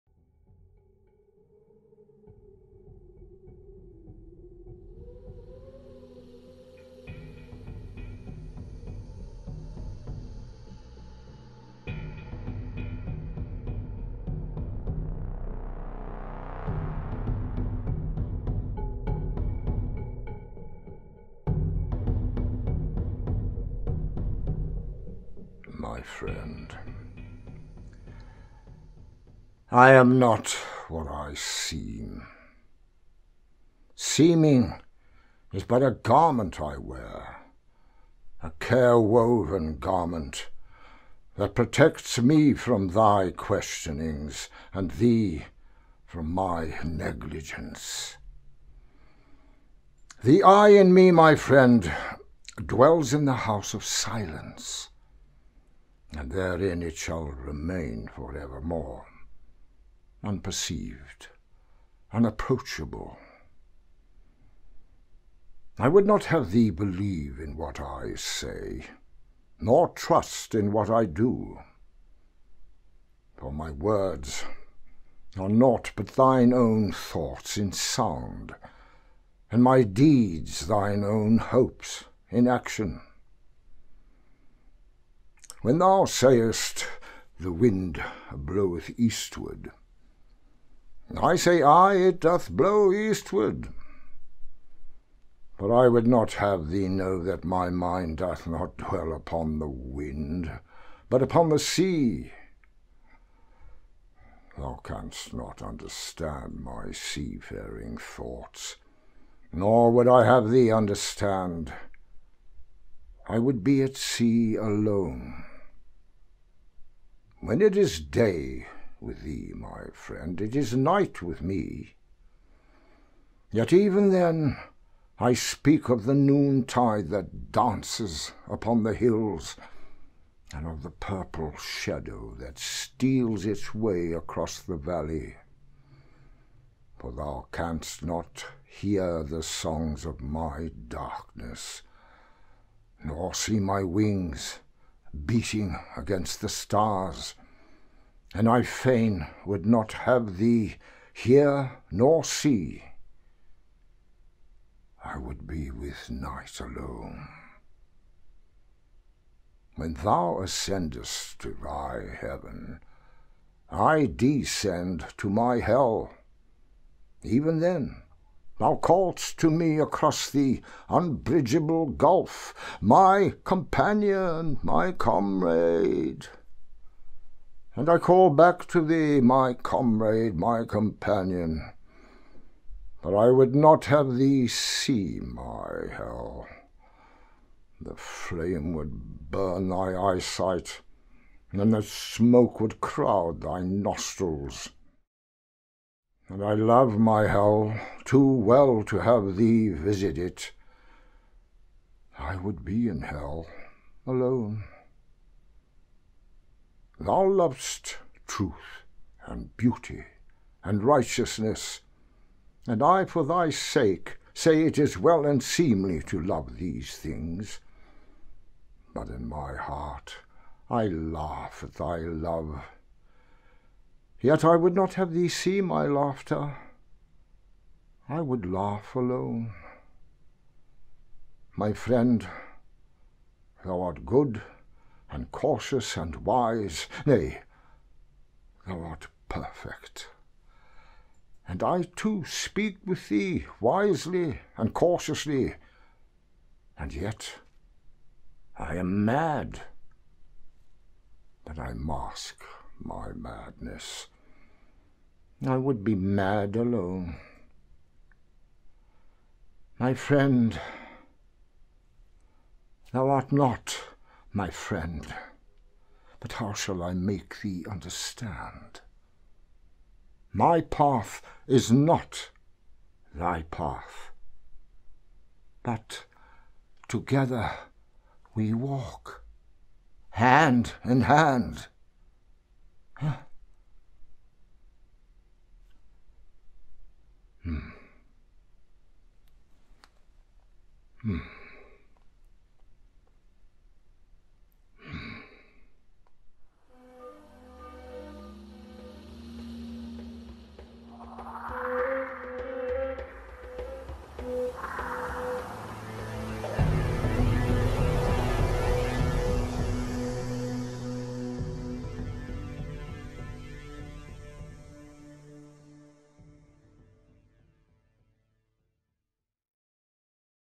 'My Friend'- by Khalil Gibran - such a deep understanding of the human condition and it's needs!
Tennesse Williams summed it up a bit more succinctly but without the poetic profundity - He simply said ""we are prisoners in our own skins .

Gibran is trying to express something sublime ...difficult if not impossible to decribe the sublime - in fact I was put firmly in my place once by a professor who explained that something "sublime" is beyond explanation - bit like God really - you may know it or feel it but not describe it and as the Bhudists would have it ..don't bother trying ... Here you sure can feel it!...